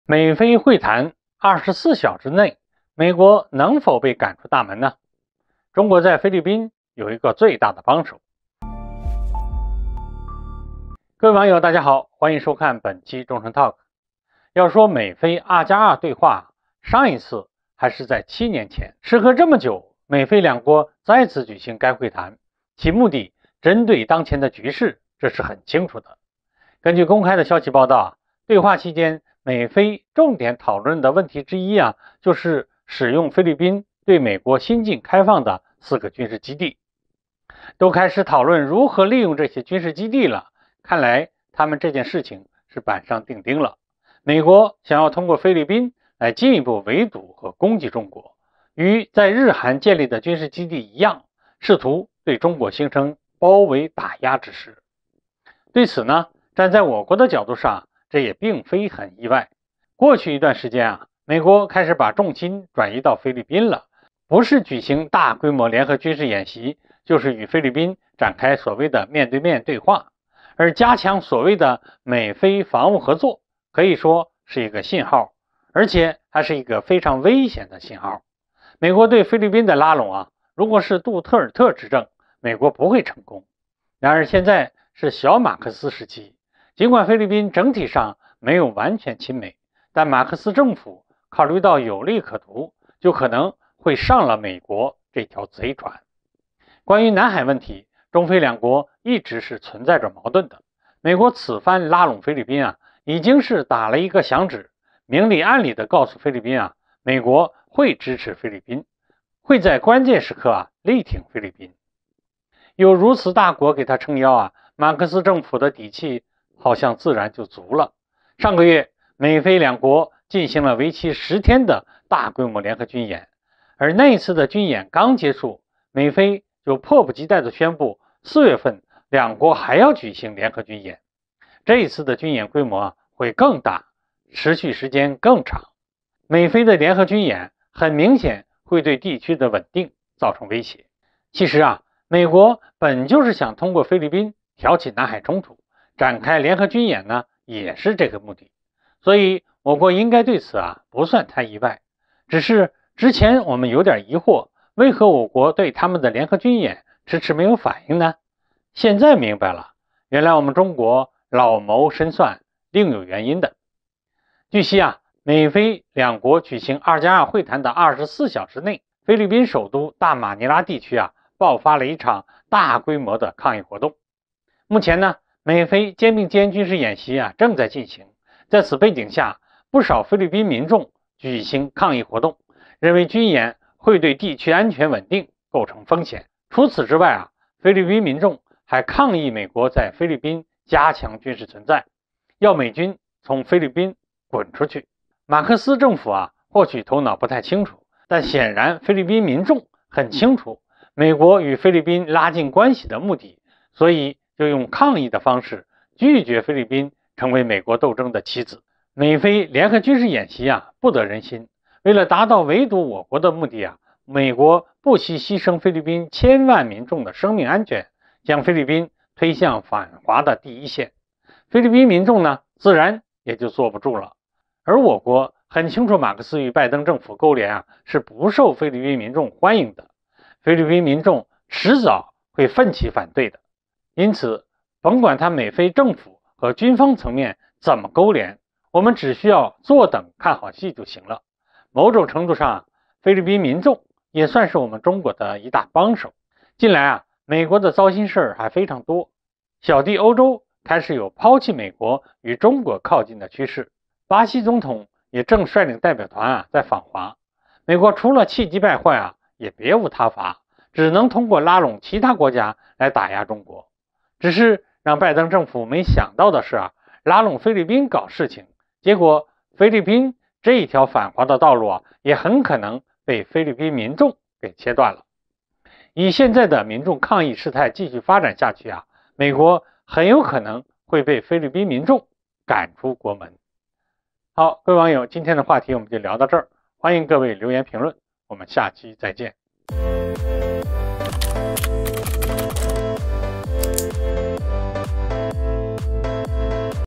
中國靜待其變，一場大規模抗議在菲引爆，美國最後一條後路要斷了【卢忠诚】
美菲會談24小時內，美國被趕出大門？中國在菲律賓有個最大幫手
中國靜待其變，一場大規模抗議在菲引爆，美國最後一條後路要斷了
防务面对面 軍事 卢忠诚 我們的影片會遵循公平、尊重的原則，確保不會包含任何仇恨言論的元素。